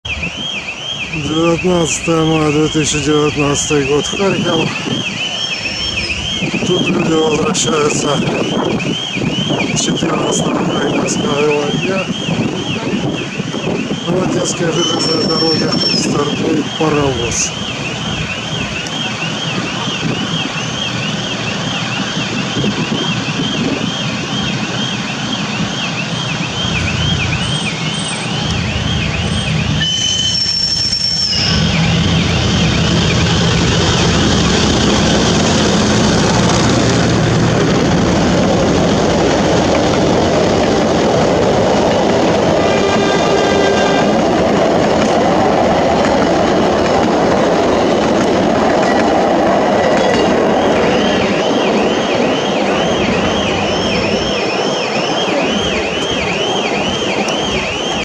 Детская железная дорога в Харькове 19 мая 2019 старт поезда малой ЖД
От вокзала Детской железной дороги в Харькове отправляется поезд 19 мая 2019 года. По малой железной дороге движется миниатюрный поезд.